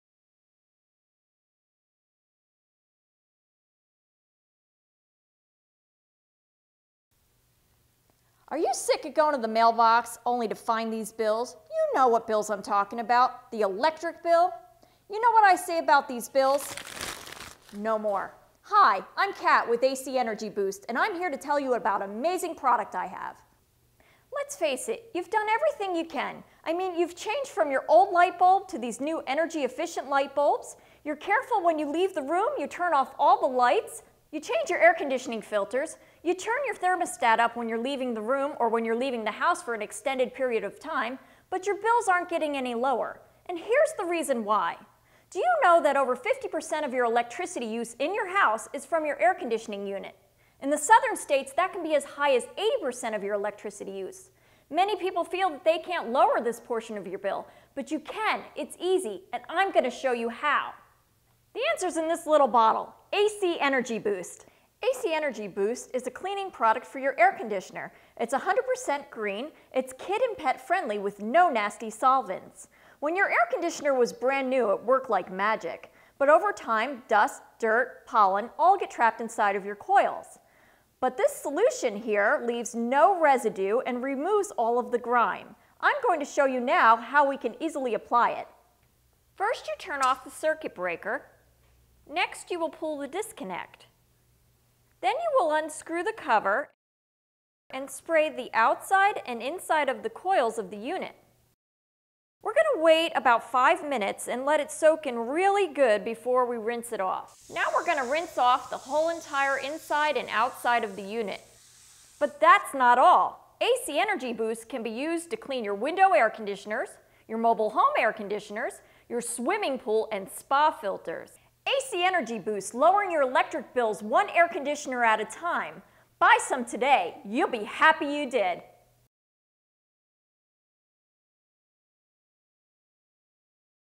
Save Money On Power Bills
AC-ENERGY BOOST will Save Money on Power Bills. It cleans the evaporator and condenser coils. Clean coils mean that the AC runs cheaper and lasts longer. It Will Lower your Electric Bills